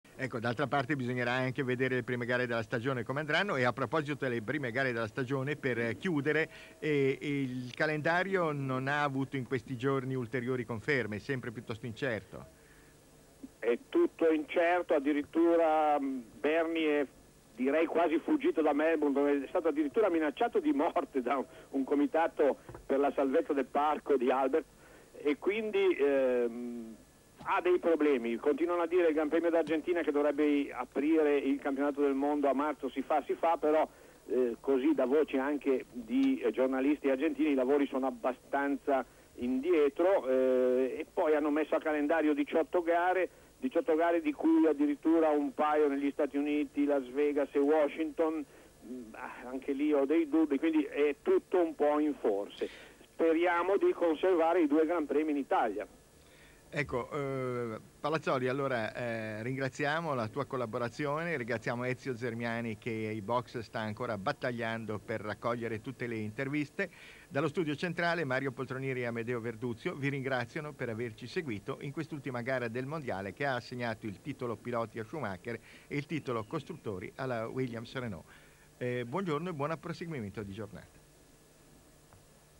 F1 Australia 1994 - Ultimo minuto di commento di Mario Poltronieri
Da Rai 2.